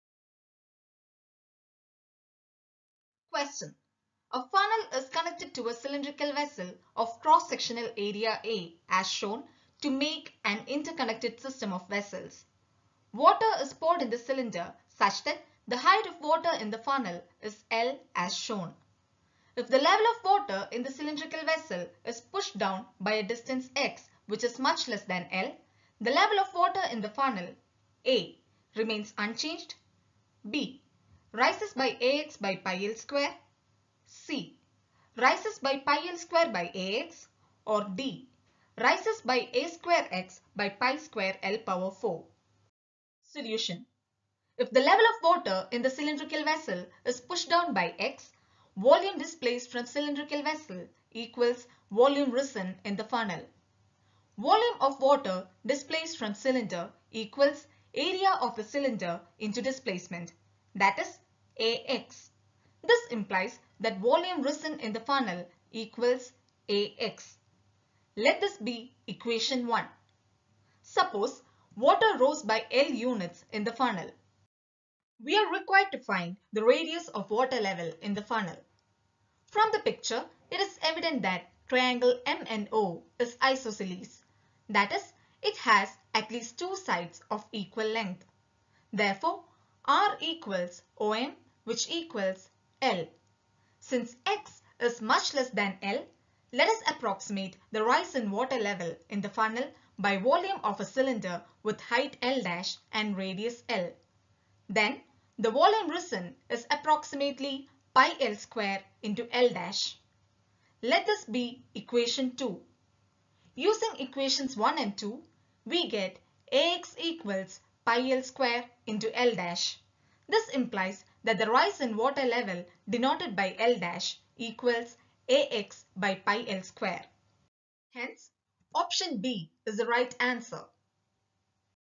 A question on Water level | CSIR-NET Dec 2018 | General Aptitude | July Mary Mathew
This is a  Part-A question of December 2018 CSIR-NET (MATHEMATICAL SCIENCES, CHEMICAL SCIENCES, EARTH SCIENCES)

Question : A funnel is connected to a cylindrical vessel of cross sectional area A as shown, to make an interconnected system of vessels. Water is poured in the cylinder such that the height of water in the funnel is l as shown. If the level of water in the cylindrical vessel is pushed down by a distance x less than l, the level of water in the funnel:
(a) remains unchanged (b) rises by Ax/xl2

Question - 0:00
Solution - 0:43
Answer- 2:08

OTHER BOOKS

OTHER BOOKS